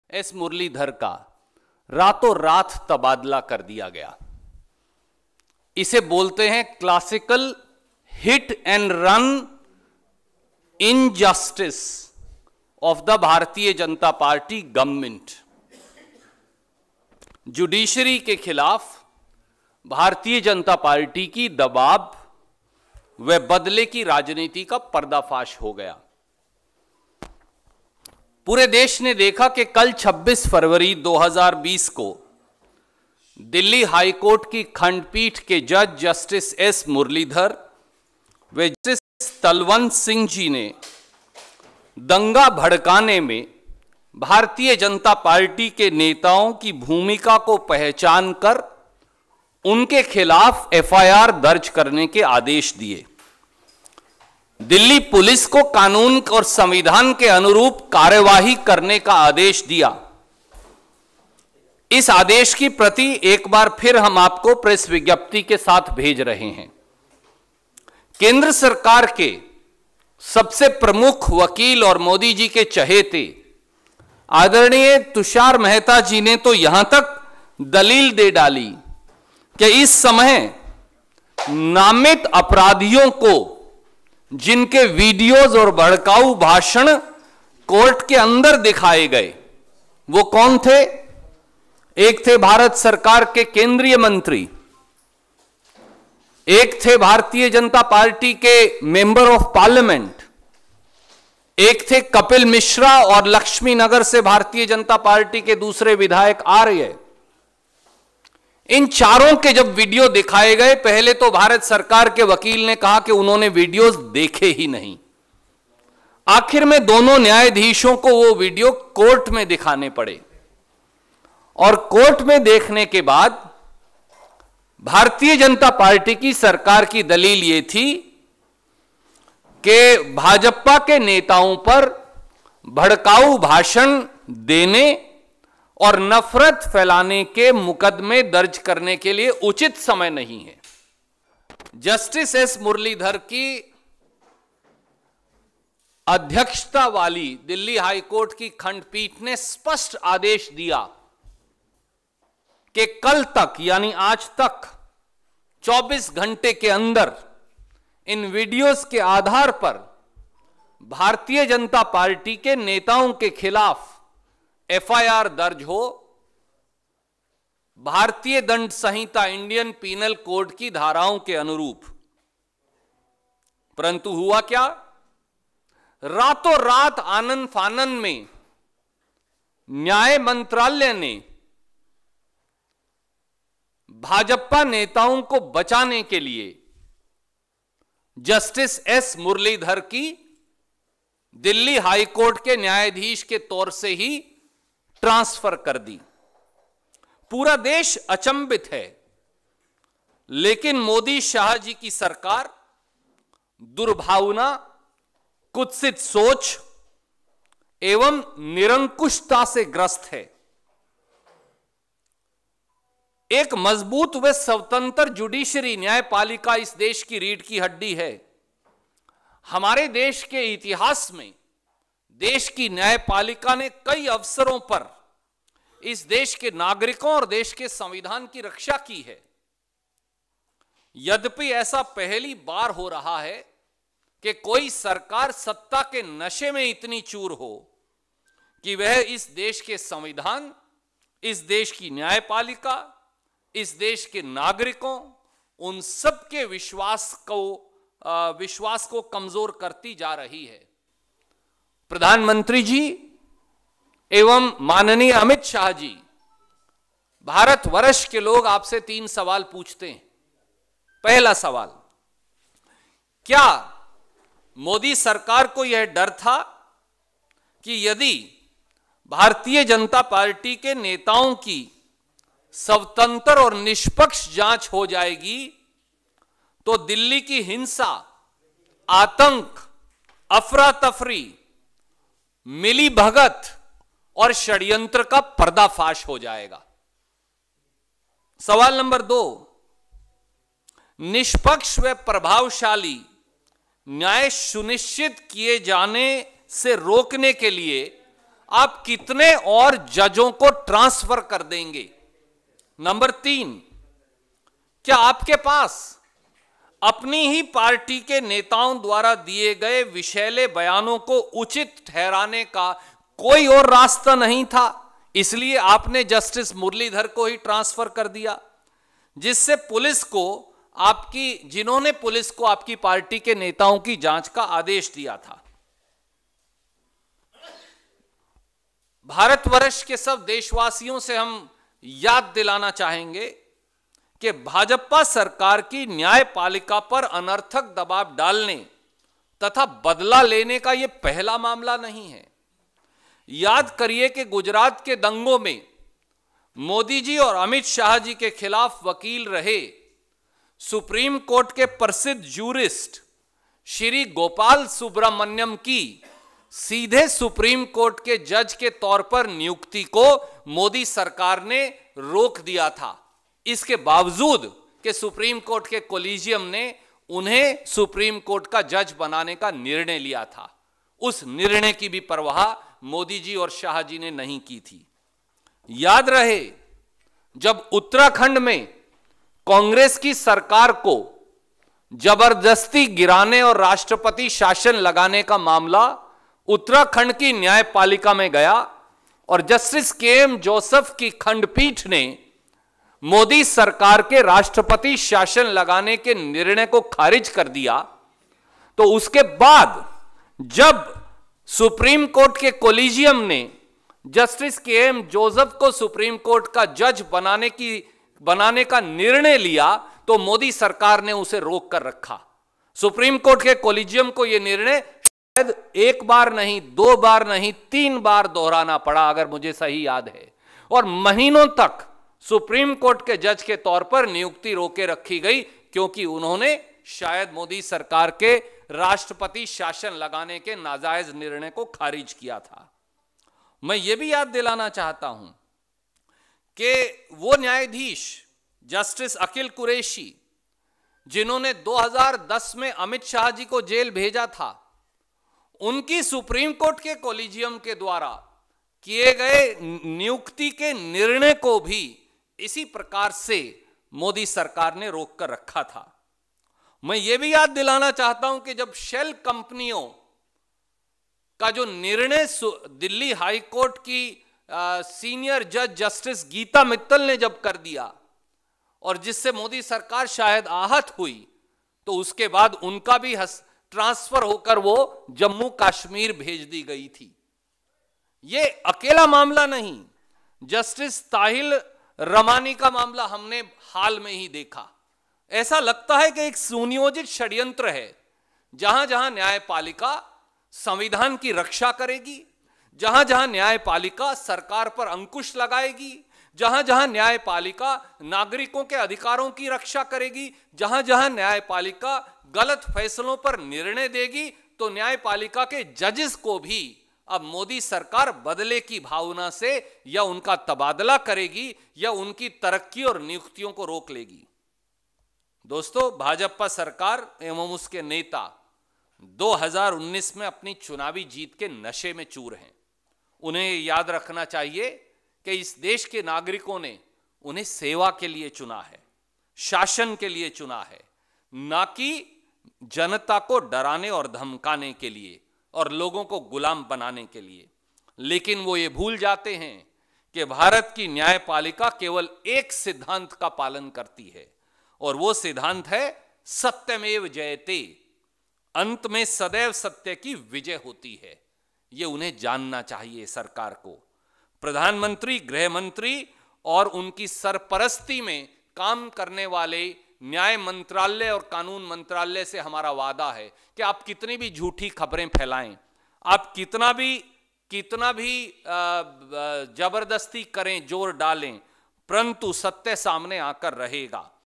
Congress का आरोप pre-planned riots' in Delhi सुरजेवाला बोले- पहले ही चेताया था
दिल्ली में भड़की हिंसा पर कांग्रेस मुख्यालय में पार्टी प्रवक्ता रणदीप सिंह सुरजेवाला ने एक प्रेस कॉन्फ्रेंस की और दिल्ली में फिर से अमन-शांति स्थापित करने की अपील की. प्रेस कॉन्फ्रेंस के दौरान सुरजेवाला ने यह भी कहा कि कांग्रेस ने पहले ही सचेत किया था कि कुछ लोग दिल्ली को दंगों वाली दिल्ली बनाना चाहते हैं. @republichindustan